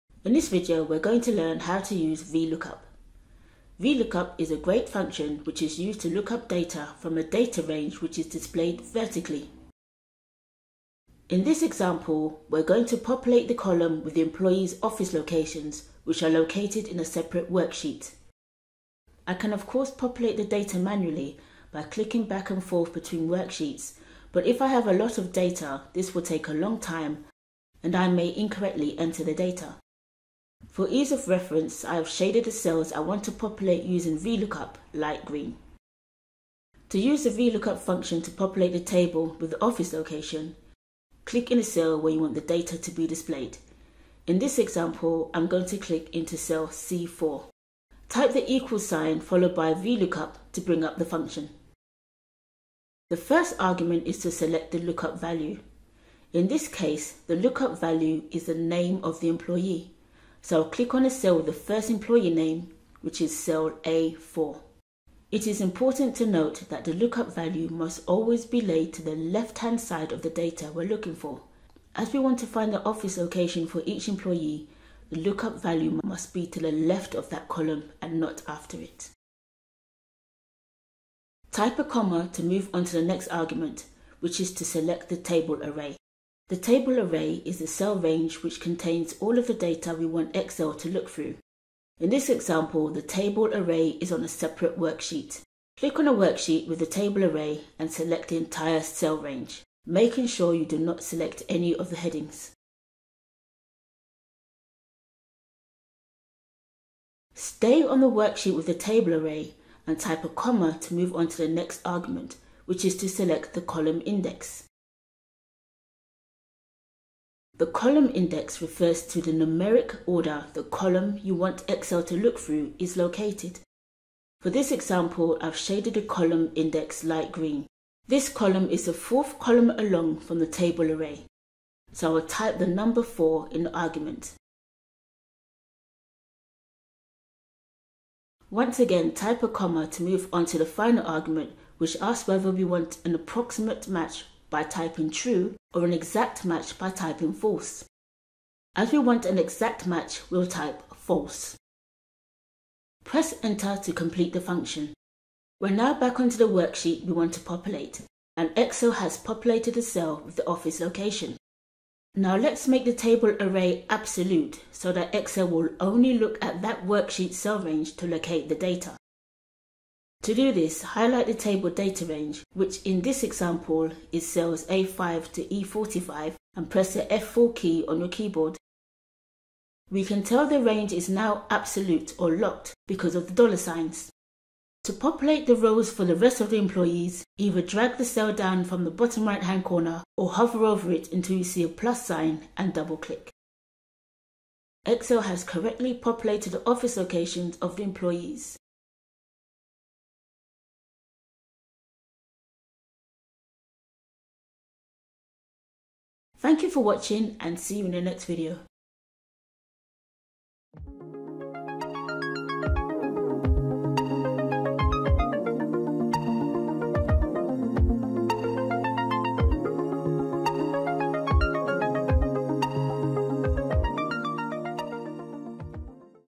How to use VLOOKUP in Excel - Excel 2019/Excel 365
Learn how to use VLOOKUP in Excel to quickly and easily locate data in an Excel spreadsheet